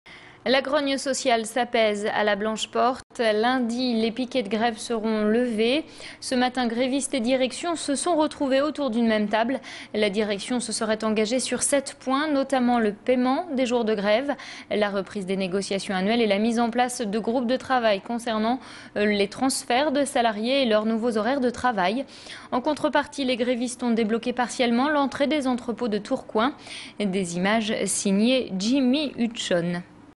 Fin de conflit à La Blanche Porte (Tourcoing)
Les salariés de La Blanche Porte ont bloqué leur entreprise de Tourcoing pour protester contre le regroupement des service logistique et relations clients prévu par les 3 Suisses et pour dénoncer une réorganisation du temps de travail les obligeant à travailler pour certains le samedi. Un accord ayant été trouvé avec la direction, les salariés grévistes reprendront le travail dès lundi

Sites webs/blogs, copiez cette vidéo avec le bouton "Intégrer" !